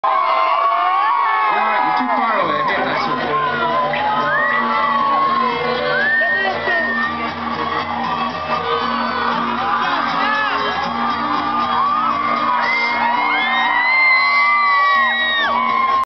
Niall Breslin - The Blizzards
Bresy from The Blizzards at UL Rag Week 2009